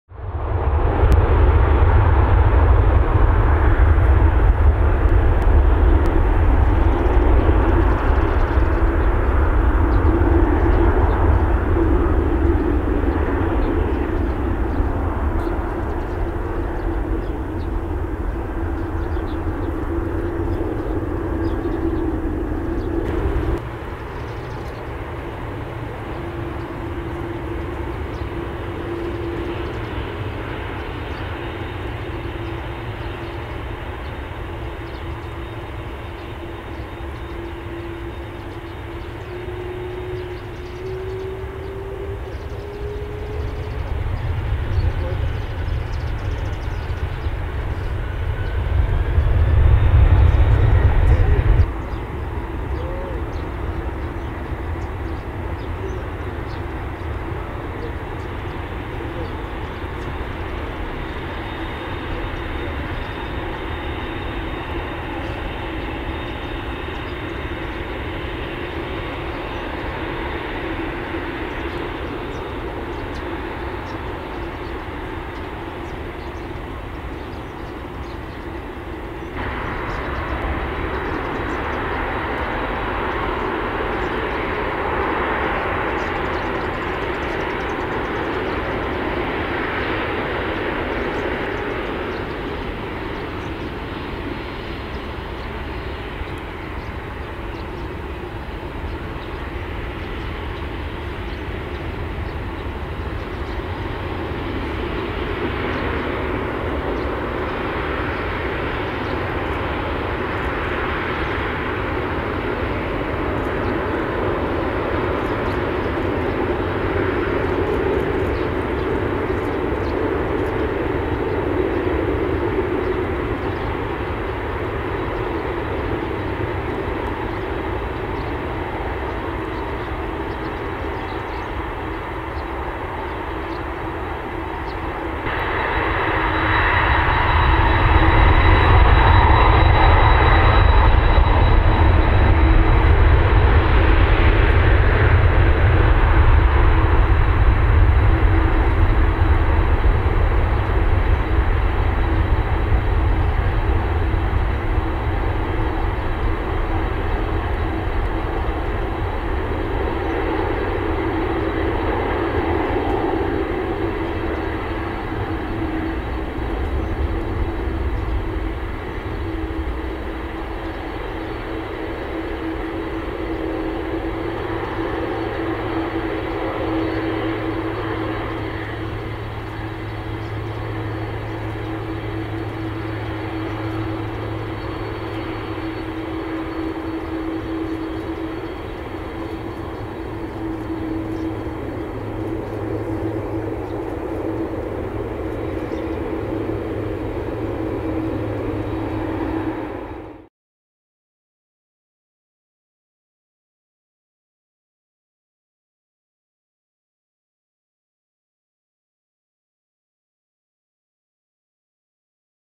Boeing 747-8KZF/SCD Nippon Cargo Airlines JA13KZ Narita Airport RWY 34L
Takeoff!!!
日本貨物航空 - Nippon Cargo Airlines [KZ/NCA]
Boeing 747-8 (Boeing 747-8KZF/SCD)
JA13KZ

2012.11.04